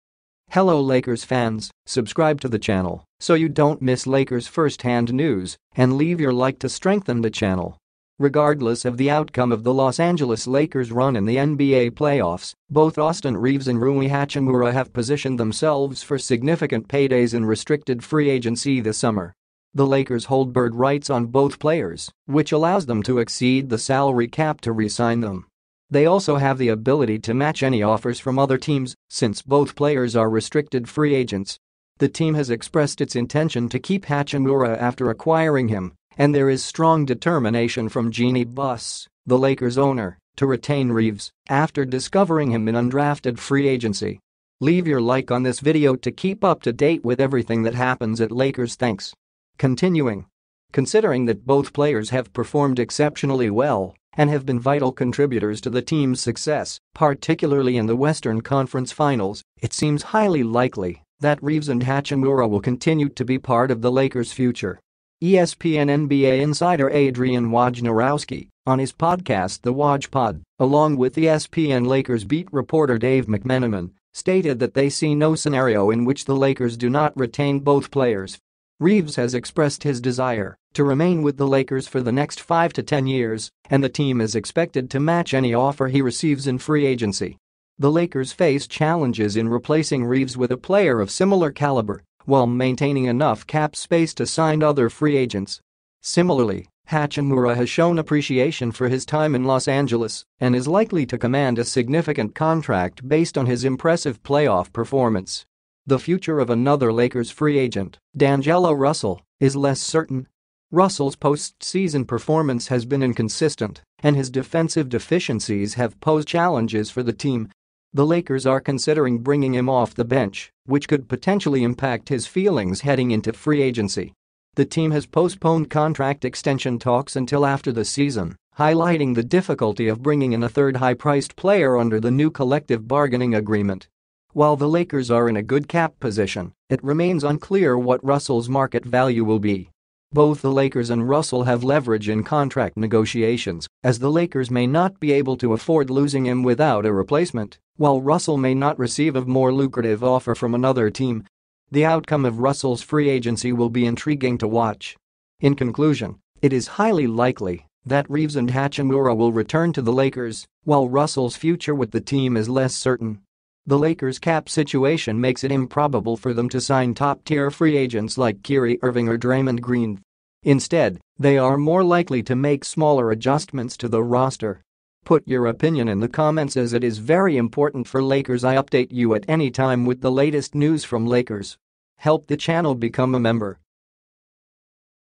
LAST HOUR! DARVIN HAM OF REAVES-HACHIMURA-RUSSELL UPDATE! LOS ANGELES LAKERS NEWS TODAY
The Lakers are one of the most popular teams in the NBA. Today, fans are eager to see the Lakers game that takes place later. The latest Lakers news suggests that they are preparing well for tonight's match. In addition, there are many news about the Lakers coming out today, including updates on injuries and changes in the lineup. If you are a Lakers fan, keep an eye on the news to get the latest information about your favorite team!

PARTNERSHIPS ONLY, OTHER MATTERS I DO NOT ANSWER.